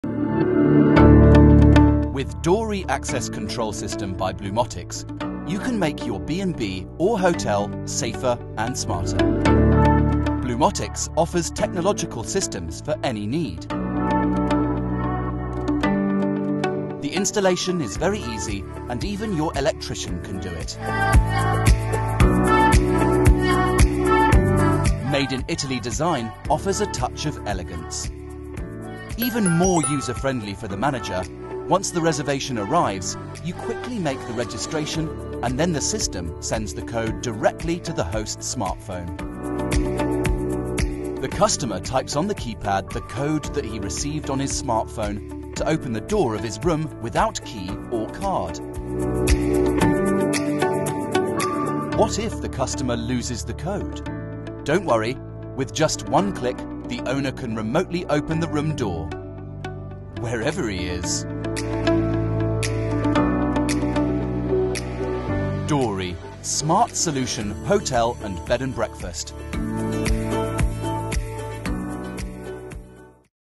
Doory, the smart lock
Doory is the new access system dedicated to hotel, bed and breakfast and tourist accommodation. This new access control system, designed and produced by Blumotix, allows the guest to enter a tourist accommodation using the credentials received via SMS.